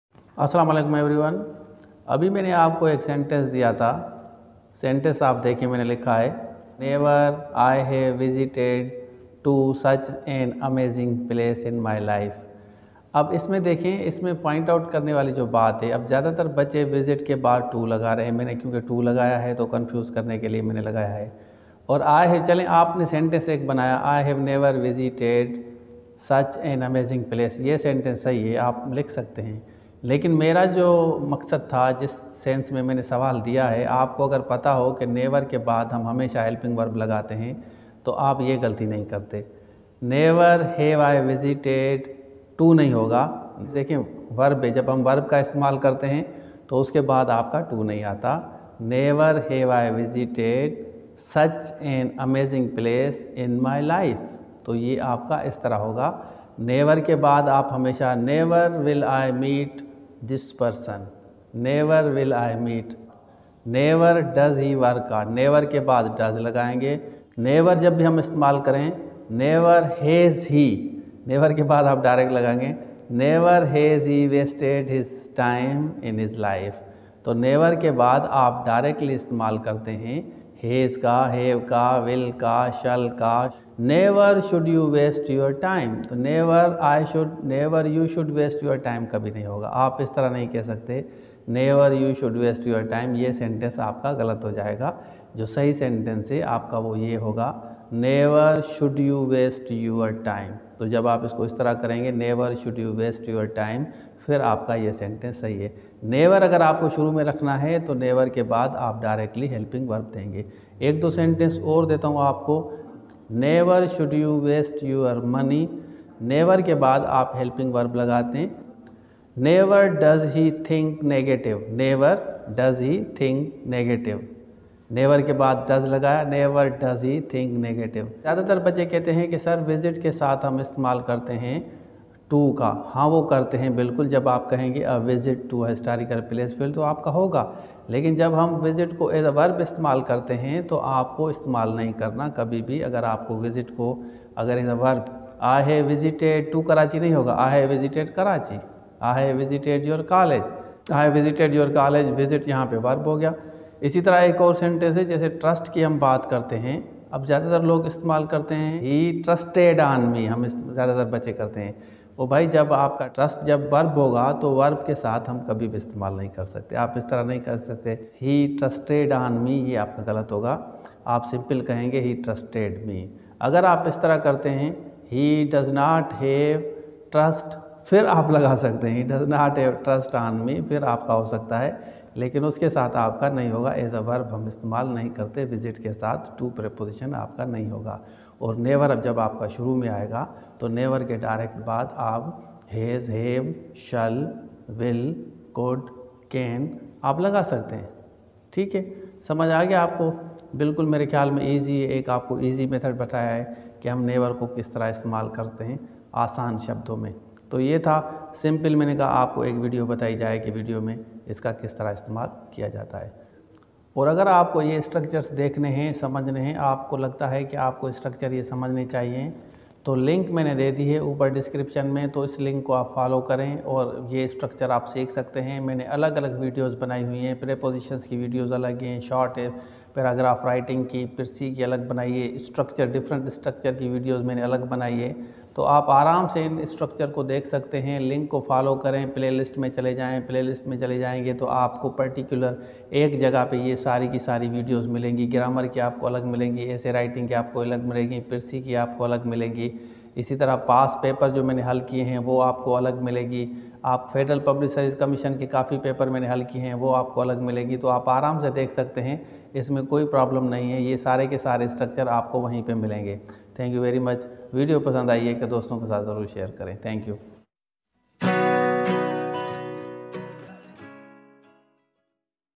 Vocabulary Problem || Used in different Sentences || Easy Trick to understand ||Prof Rasheed Mirani
Vocabulary Problem || Used in different Sentences || Easy Trick to understand ||Prof Rasheed Mirani Senior Educationist

1) How to understand vocabulary
2) How to use words in Sentences
3) Best method of understanding words
4) Verbs and their uses
5) use of Never in the beginning